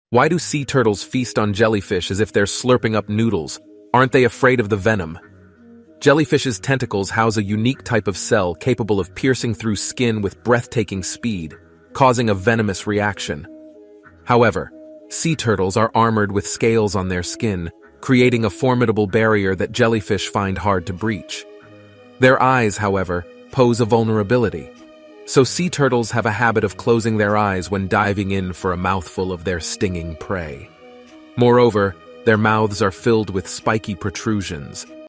Sea turtles vs jellyfish 😨
⚖️ Legal Counsel:
Geoffrey Gross

📜 Story:
In this enlightening video, we dive deep into the fascinating relationship between sea turtles and their favorite prey - jellyfish. Discover why these seemingly defenseless marine creatures pose no threat to the armored sea turtles, and how these turtles feast on them with delight. Learn about the unique anatomical features that protect the turtles from the jellyfish's potent venom, from their scaled skin to their keratin-filled mouths. We'll also explore the intriguing behavioral adaptation of sea turtles closing their eyes when feeding, ensuring their safety against these stinging creatures. Join us as we unravel the mysteries of the marine world and highlight the extraordinary resilience of sea turtles in their quest for survival.